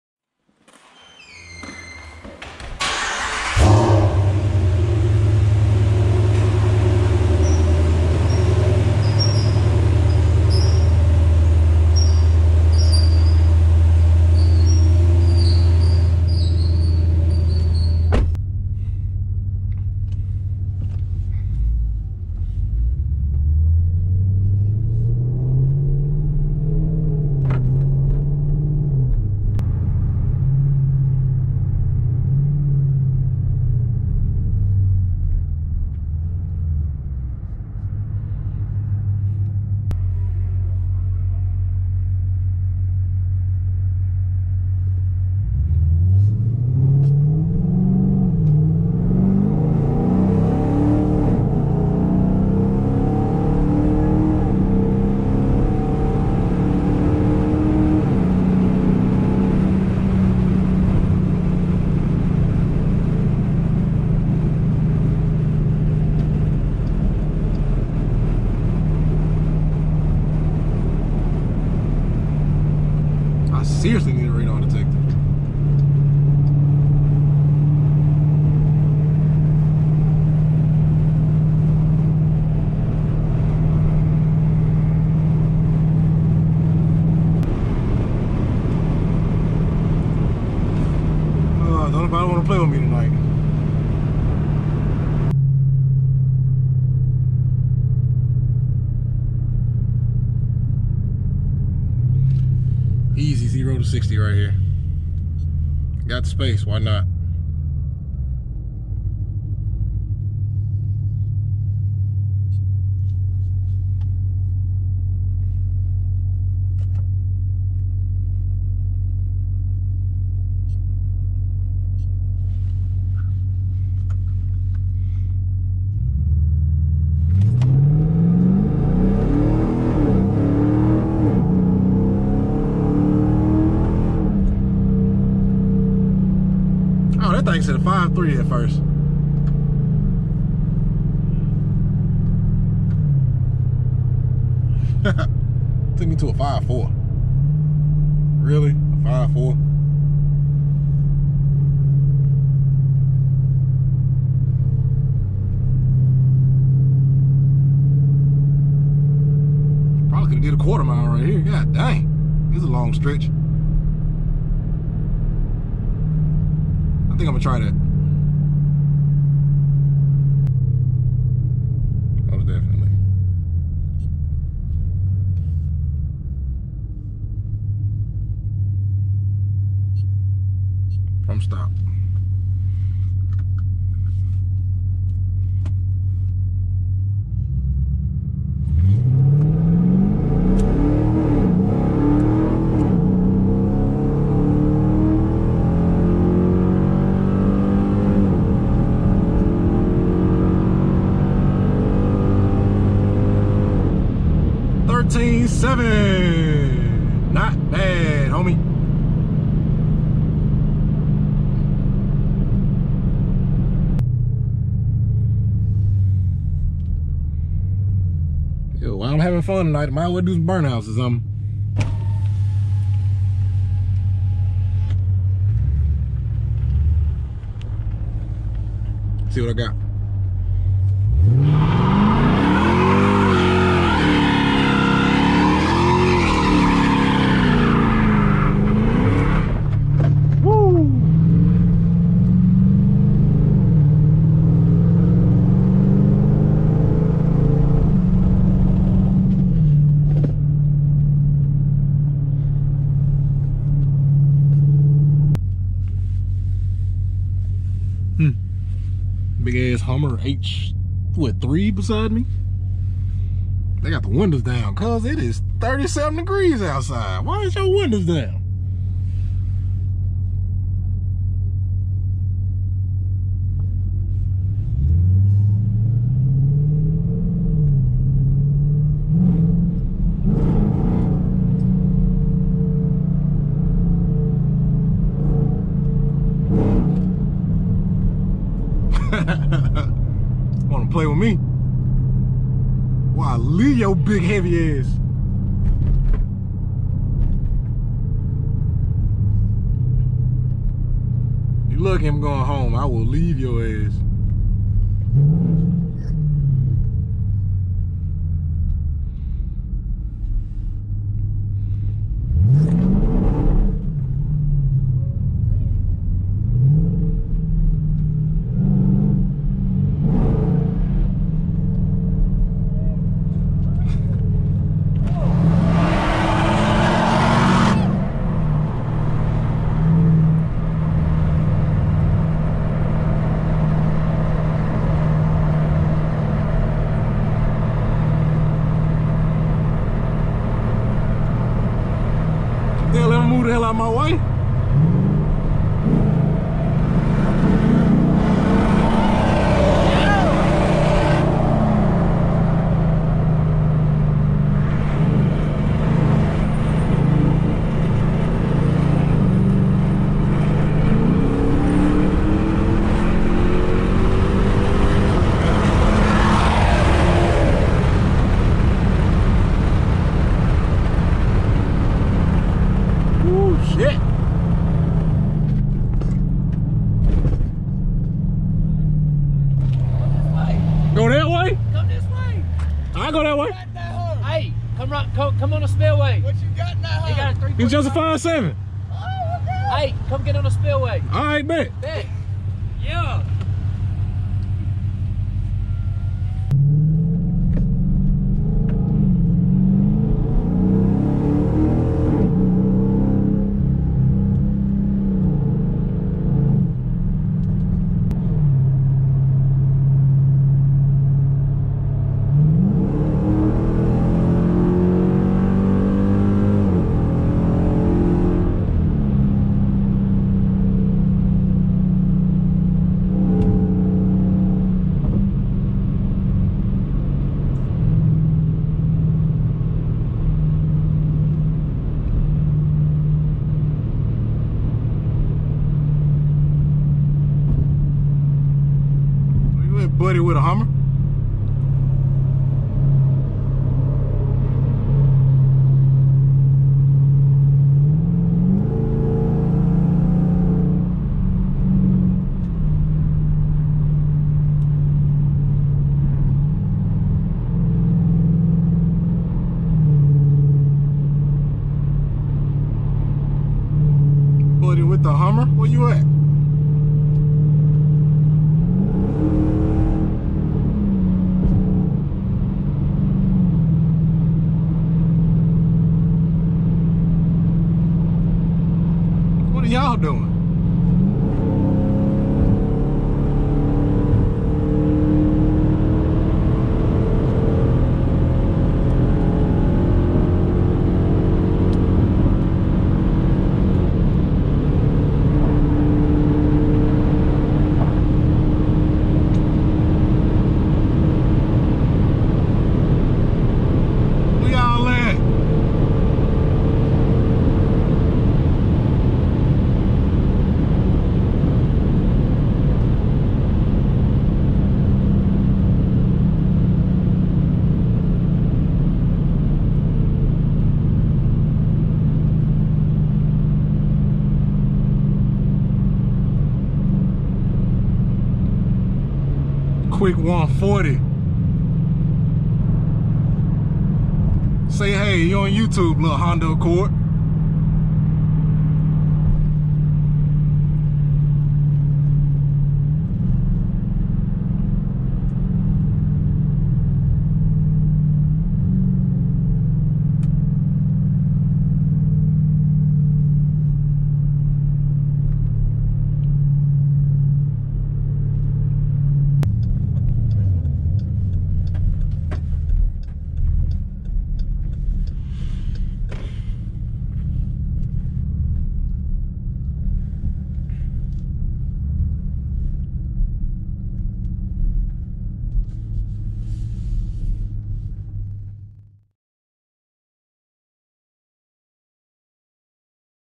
Looking For Trouble Pt.1/ Durango goes 140mph
In my first episode of "Looking for trouble", I take to the highway to see if i could find some races. I ended up running a 0-60(5.3) and a 1/4 mile13.7@ 103mph). After that I  began to head home when i finally found the trouble i was looking for.